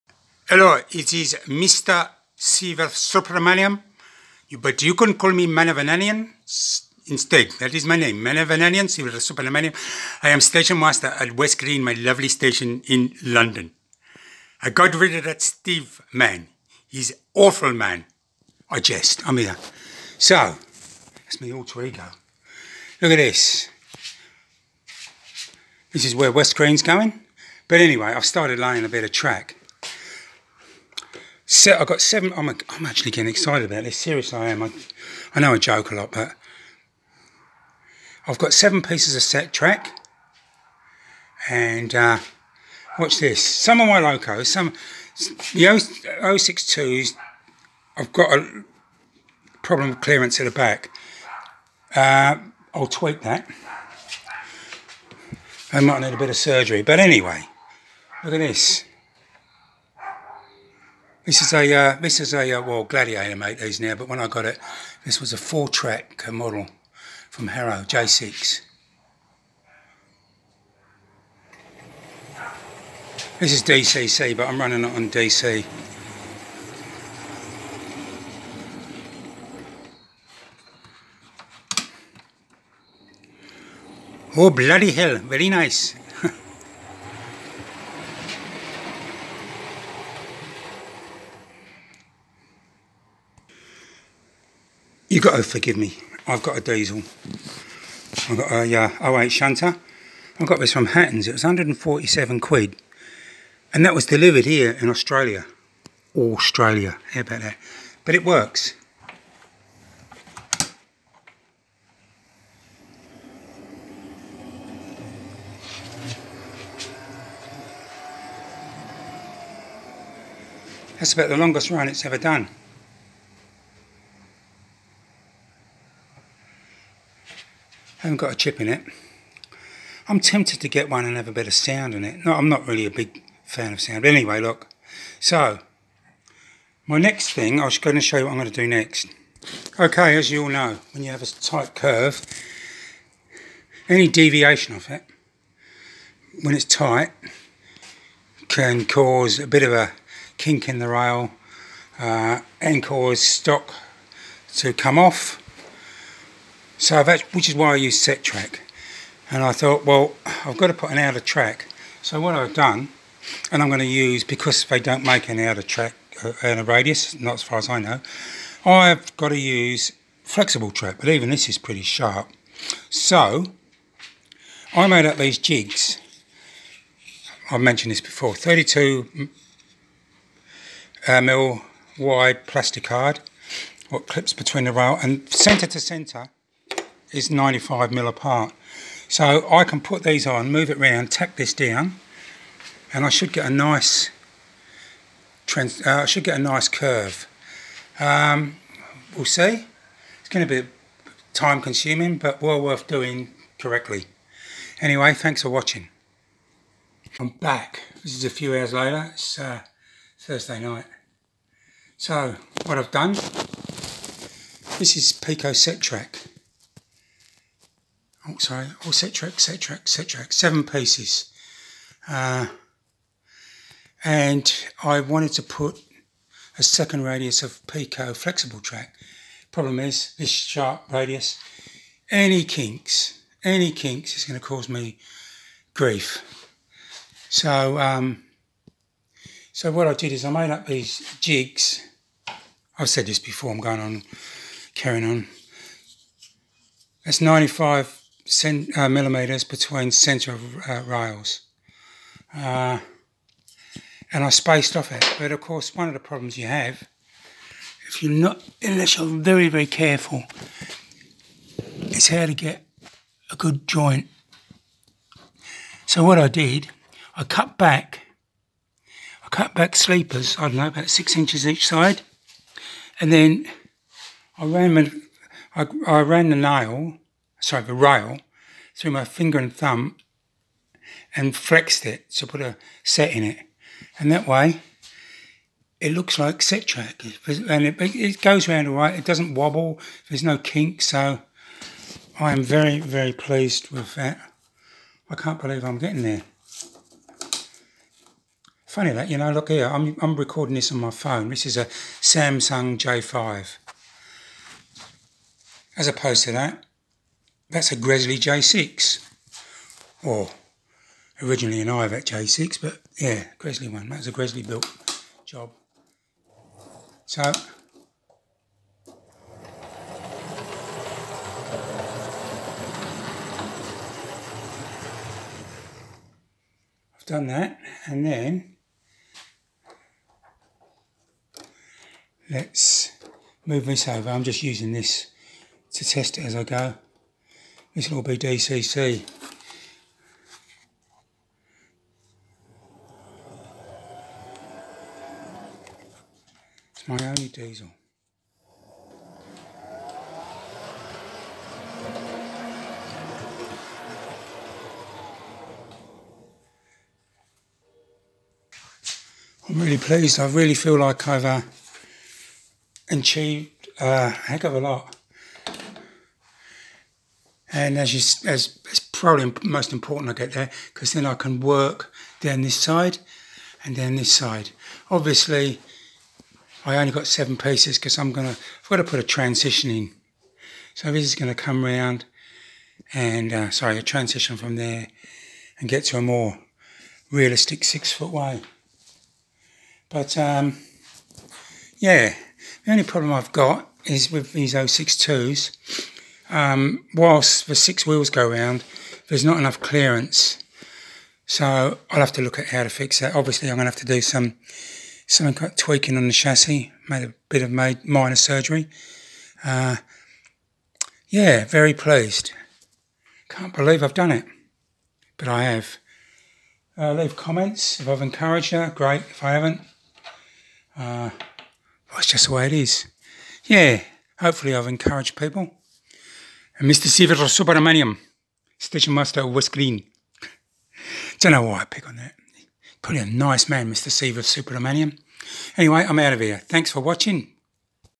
West Green 0 gauge update 12th September 2019
Some track laid and tested Woohoo Choo choo